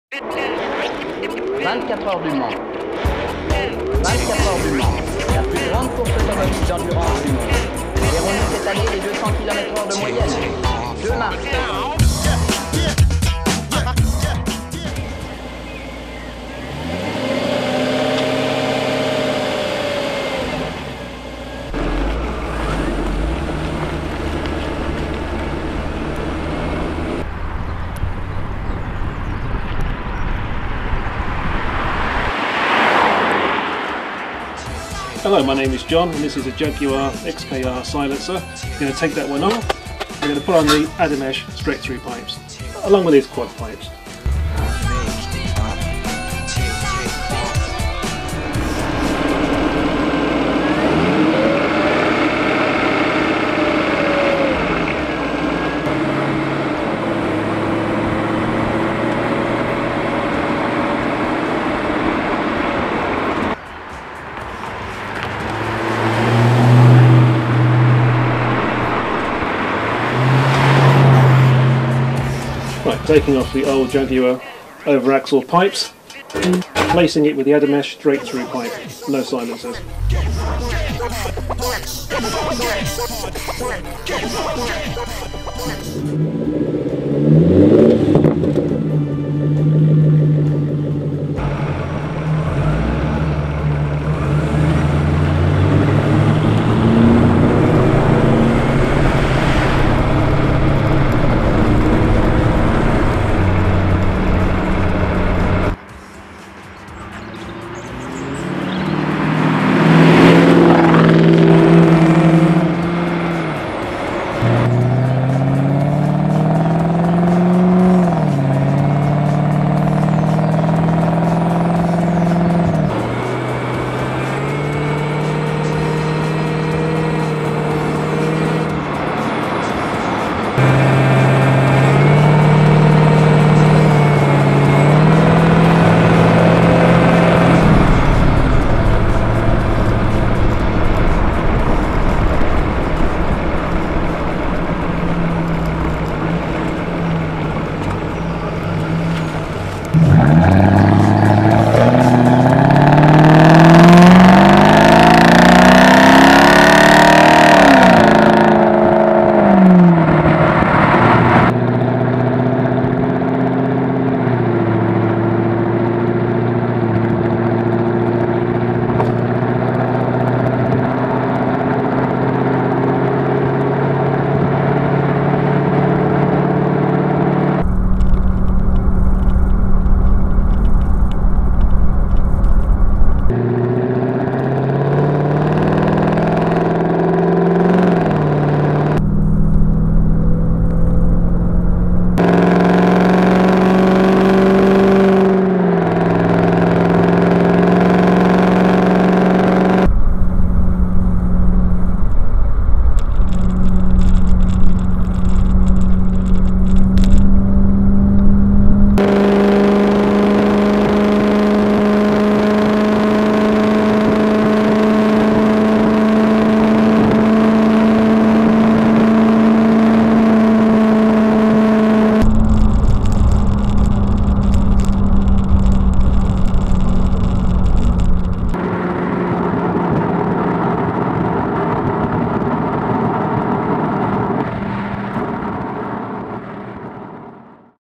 Adamesh Jaguar XKR v4
Independent test on the Adamesh Exhaust upgrade for the Jaguar XKR 4 (and 4.2L) versions from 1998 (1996 XK8) to 2006.  Two stages, a popular choice is the quick DIY job to remove the old rear silencer pipes replacing with the Adamesh straight through section either with the original type Oval or Quad tailpipes, made from Stainless Steel, original 2 inch pipe.  The Over Axle sections can also be replaced, again swapping silencer old pipes with the crushed restricted bend with no crush bend straight through SS 2 inch pipes.  In this video I film the 4L XKR before conversion, which makes no engine noise what so ever, just tyre noise, then replace the back section which is a mild improvement.  Record, then add the new Over Axle sections which removes a total of four old silencers with new straight through SS pipe leaving the original large mixing silencer to prevent excessive noise.  Record.  I'm pleased with the both sections, it's not too loud, more enjoyable to drive, blind people hear the car approach and so don't get run over as frequently the case with hybrid cars, I'm just wondering if it could be customised further for added fun, but a risk of causing drone, something I don't find with this at all. In this video I also explain briefly and show the new pipes, finished with dashcam recordings of the speedo and tacho gauges.  Video, fitting and copyright JSPV.